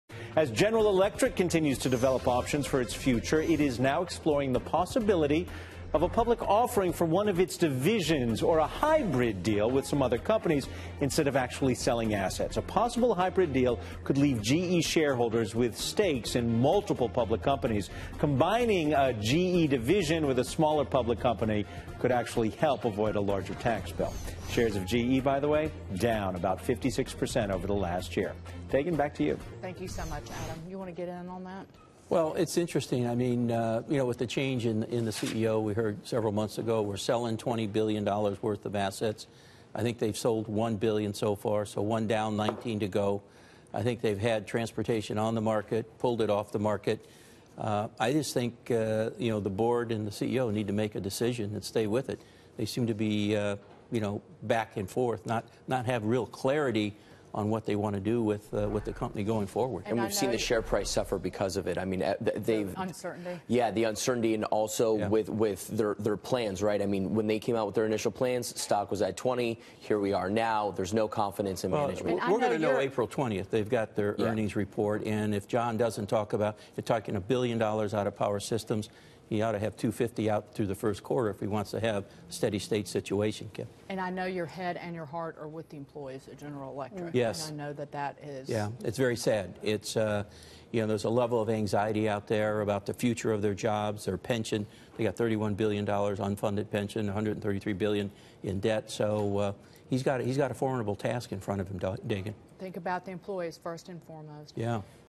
GE Board, CEO don't have clarity on what they want to do: Bob Nardelli
Former GE Transportation CEO Bob Nardelli on the outlook for General Electric exploring potential hybrid deals or public offerings of a division.